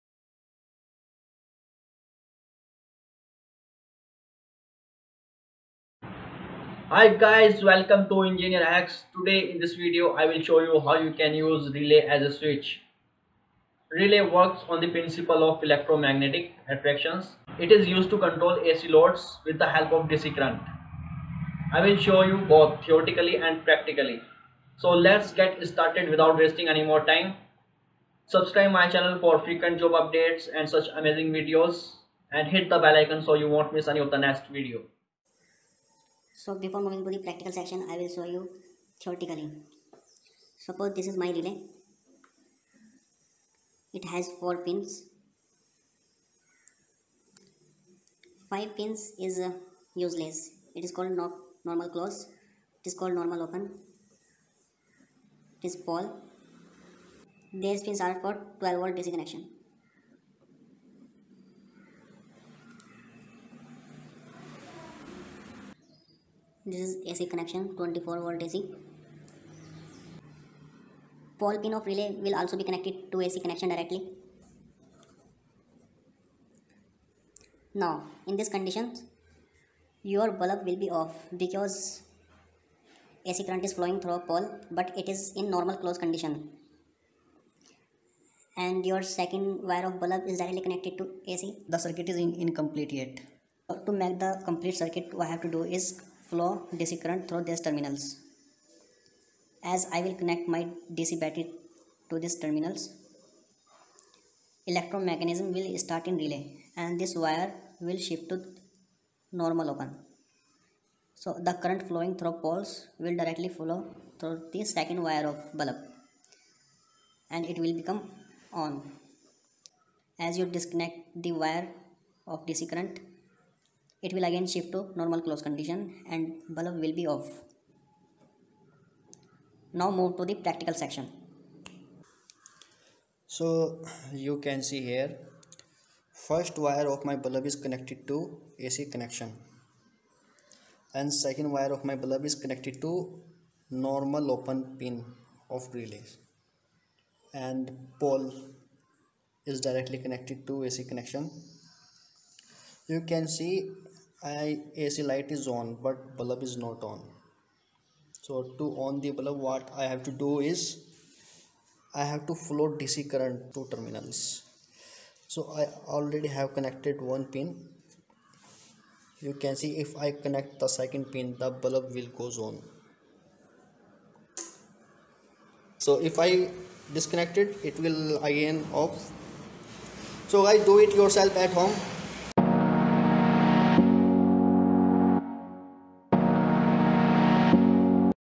Relay as a switch || Control 240V AC Bulb using Relay || How relay works?
Hi Guys,

In this video I will be telling you  how you can control 240v ac bulb or load with the help of relay.

How does relay works?
Relay as a switch?